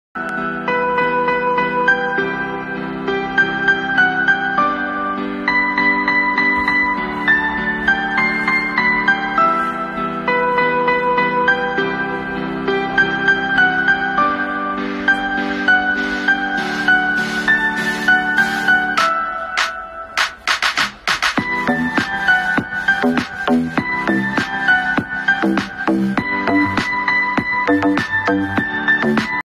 Smart Wireless Charger Atmosphere Lamp - Bedside Lamp with Bluetooth Speaker with FM, Night Light
Smart Wireless Charger Atmosphere Lamp - Bedside Lamp with Bluetooth Speaker with FM, Night Light Wireless Charger for Phone, Dimmable Colour Changing Table Lamp, with voice activation on/off etc for home, office and gifting use*